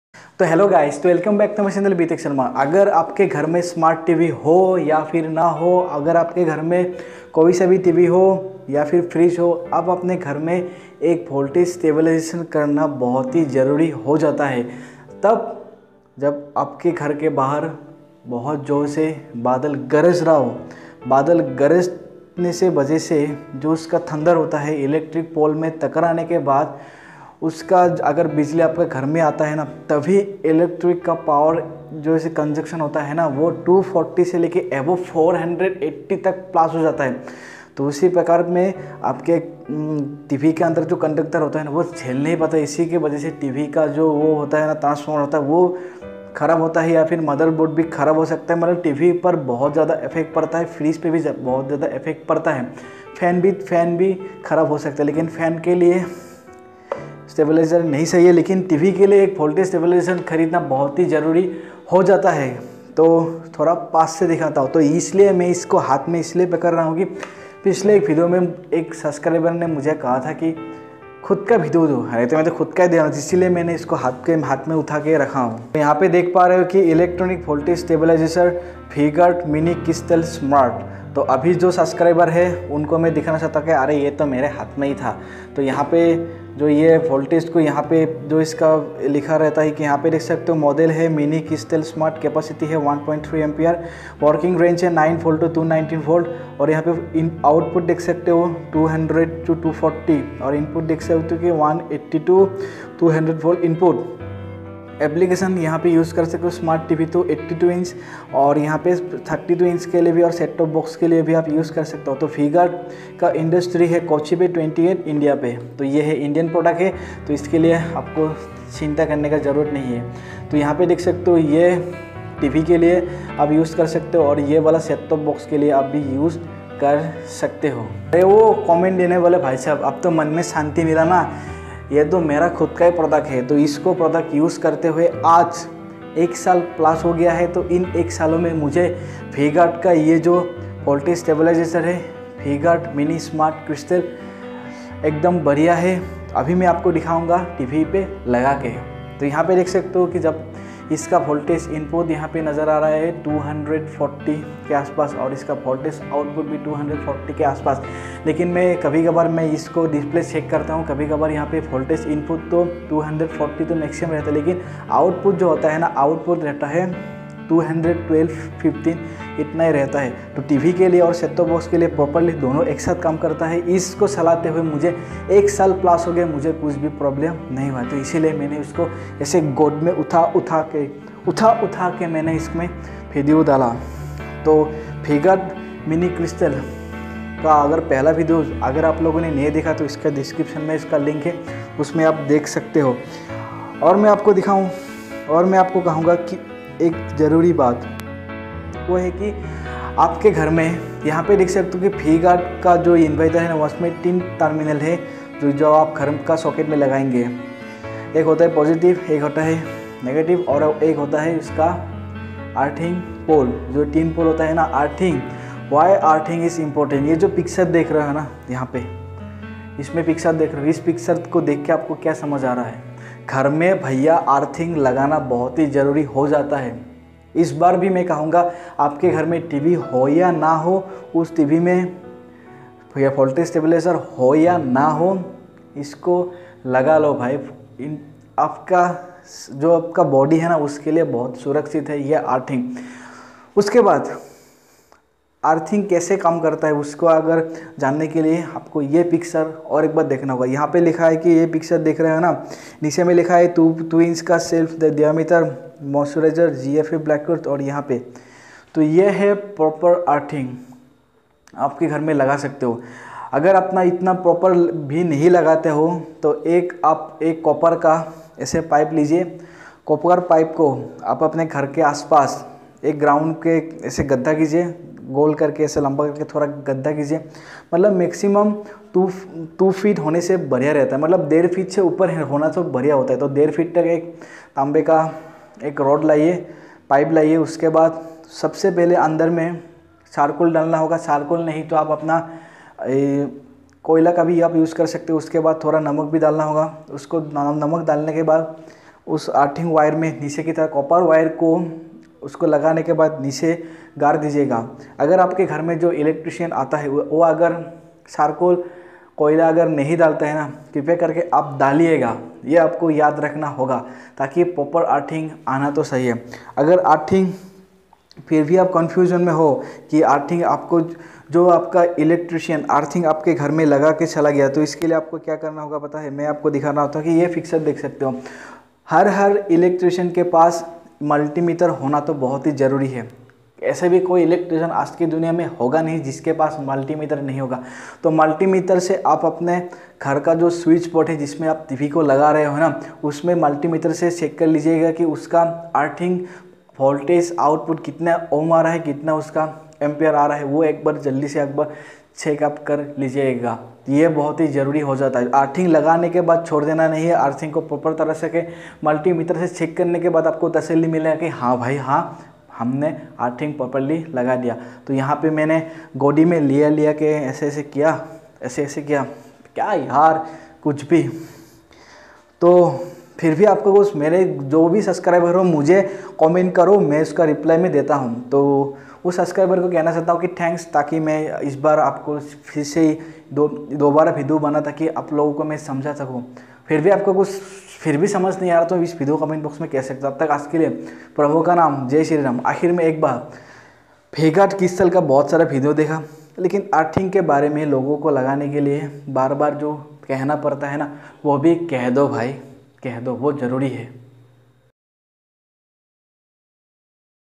V Guard mini crystal stabilizer for led tv | 1 year used Honestly review
V-Guard Mini Crystal Voltage Stabiliser for up to 32-inch TV
यह वी-गार्ड मिनी क्रिस्टल वोल्टेज स्टेबलाइजर 32 इंच के TV के लिए आदर्श है।

आप इसे बड़े TV के लिए उपयोग नहीं कर सकते। इस स्टेबलाइजर में कई आउटलेट हैं, जिससे आप अपने डीटीएच सेट-टॉप बॉक्स और डीवीडी प्लेयर कनेक्ट कर सकते हैं।

Amazon.in पर कीमत देखें
यह 90 से 290V की वोल्टेज रेंज में कार्य कर सकता है। यह स्टेबलाइज़र एक स्वचालित कट-ऑफ फीचर के साथ आता है । इसमें अन्य उल्लेखनीय विशेषताएं हैं जैसे मेन्स टर्न-ऑन देरी और 95% दक्षता प्रदर्शित करता है। इस वोल्टेज स्टेबलाइजर में (wall mounted) दीवार पर चढ़ने के साथ-साथ टेबल-टॉप डिज़ाइन भी है।